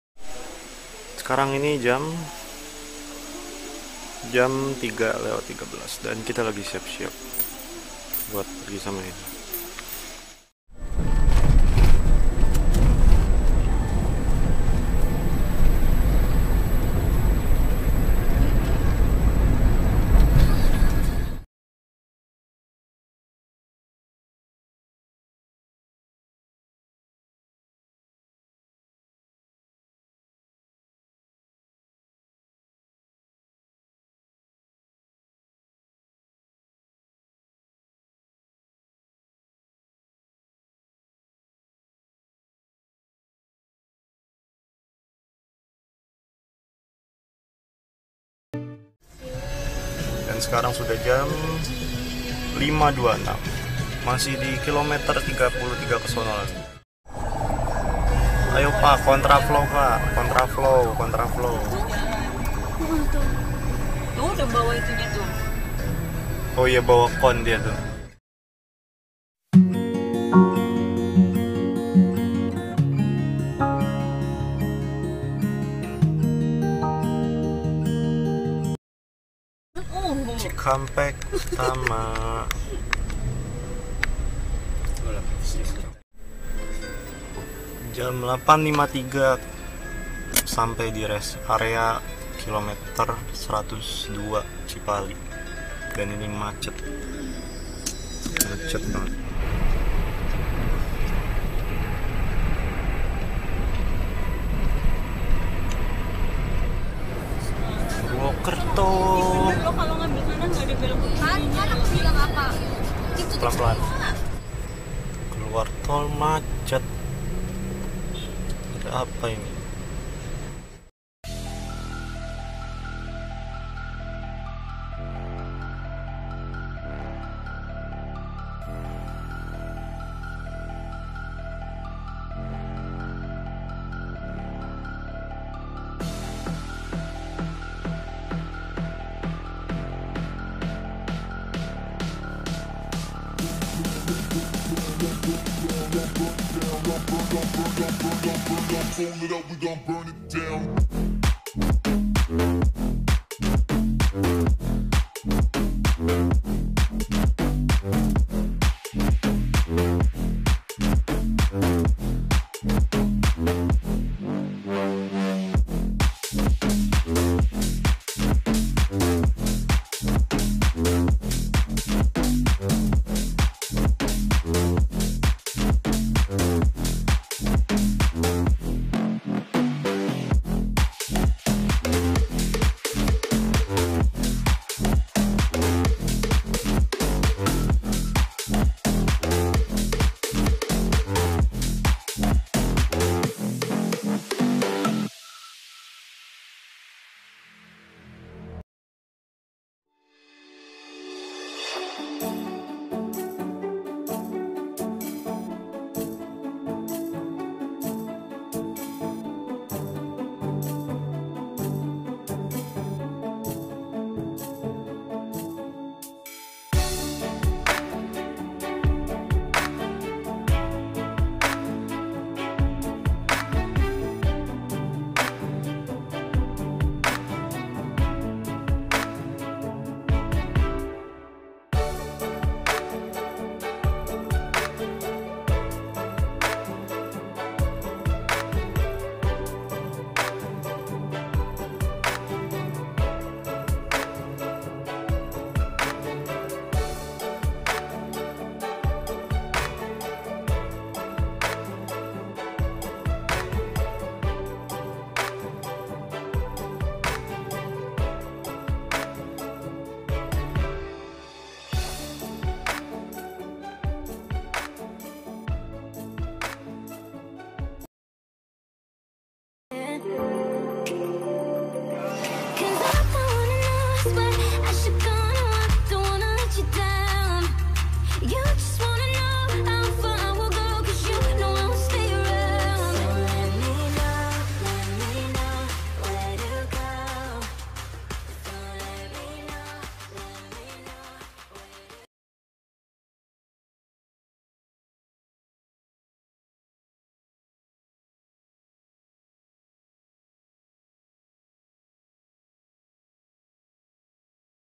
Vlog #2 : Mudik Lebaran 2019, Macet!
Video ini berisikan dokumentasi singkat mengenai perjalanan mudik lebaran tahun 2019 dari Bekasi ke Purwokerto. Perjalanan berlangsung selama kurang lebih 12-13 jam dengan rute relatif padat bahkan macet parah di beberapa sisi. Video ini juga mungkin dapat menjadi obat rindu untuk kita semua yang tidak bisa mudik di 2020 lantaran adanya pandemi Covid-19 yang terus mengintai kita dimanapun dan kapanpun. Jadi, DirumahAja ya!